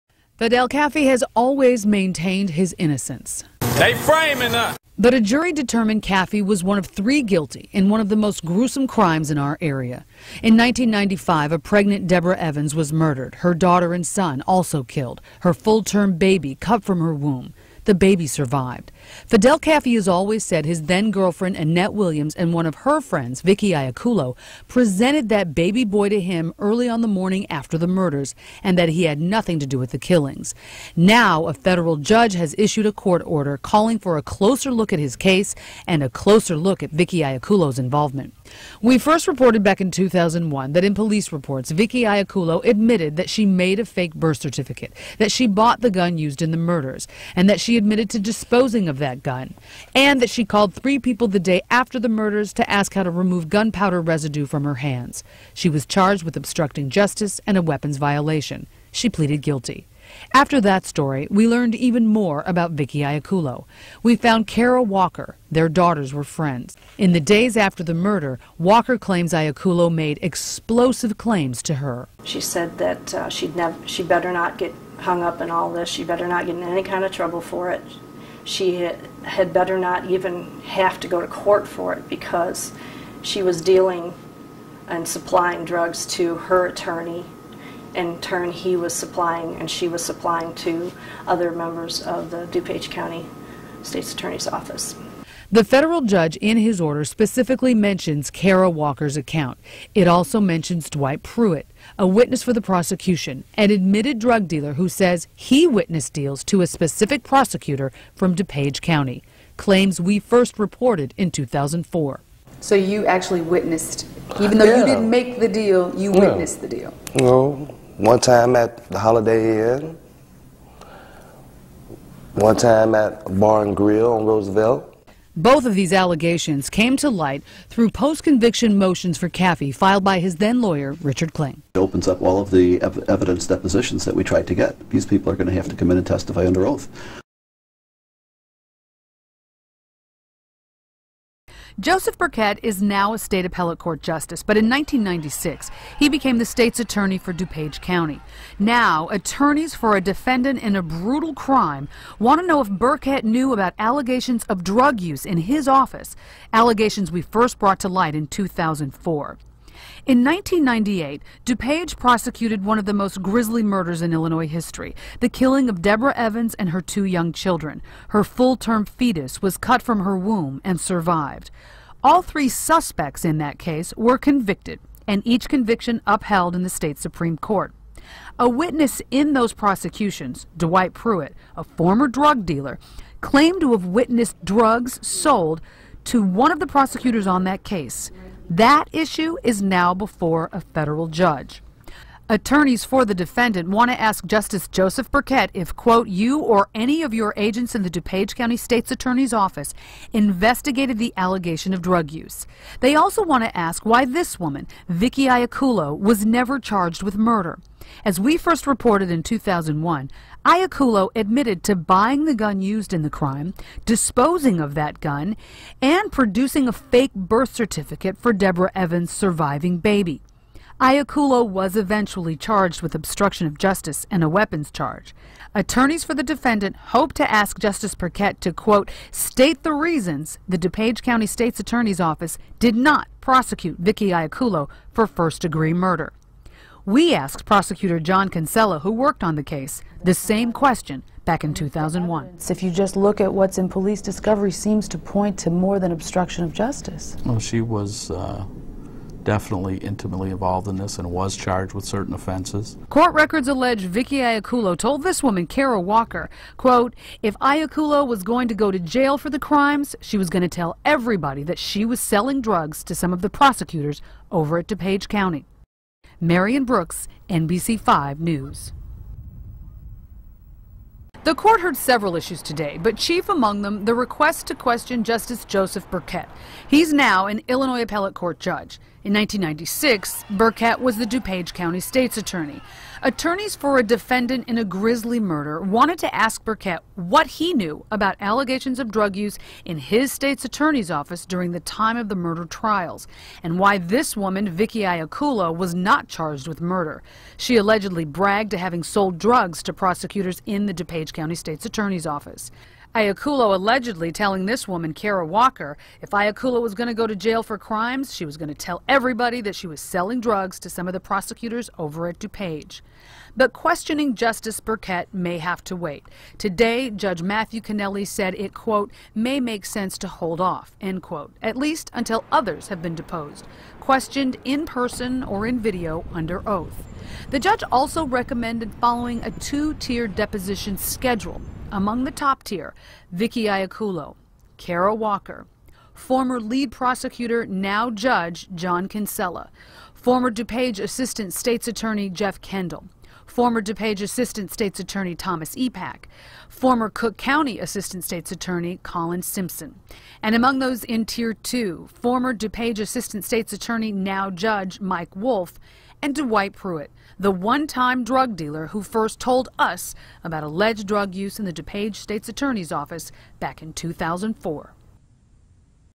01 Evans Murders Background and allegations of drug deals in Attorney's office
News story that gives some background into the 1995 Evans Murders in Dupage County Illinois that resulted in a death of sentence for two individuals and a life sentence for Levern Ward who has always maintained his innocence.   The story also briefly discusses allegations of drugs supplied to the county's attorney office by Vikki Iacullo who was intimately involved in the murder case, but was only convicted of minor charges.